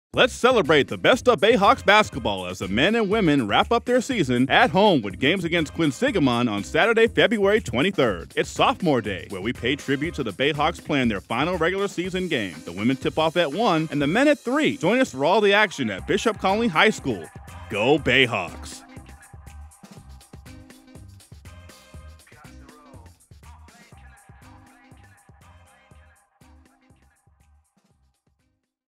Bayhawks vs. Quinsigamond Promo
Bristol Bayhawks vs. Quinsigamond Community College

Saturday, February 23rd at Bishop Connolly High School.

Women play at 1:00pm, the men play at 3:00pm.